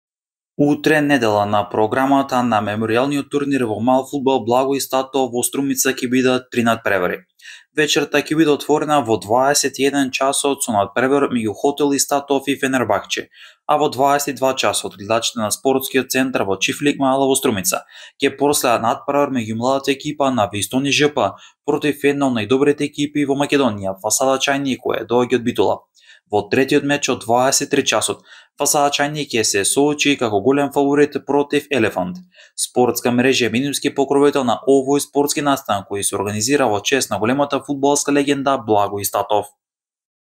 Во најава одлични и неизвесни натпревари: Фасада Чајник против Вистони ЖП и Елефант!
Утре продолжуваат фудбалските возбудувања на Меморијалниот турнир во мал фудбал „Благој Истатов“, натпреварите се играат на Спортскиот центар во Чифлик Маало во Струмица, а утре на програмата ќе бидат три натпревари.

Спортска Мрежа Podcast е посветена на македонскиот спорт каде особено медиумски простор ќе добиваат нисколигашите!

Спортска Мрежа е медиумски покровител на овој спортски настан!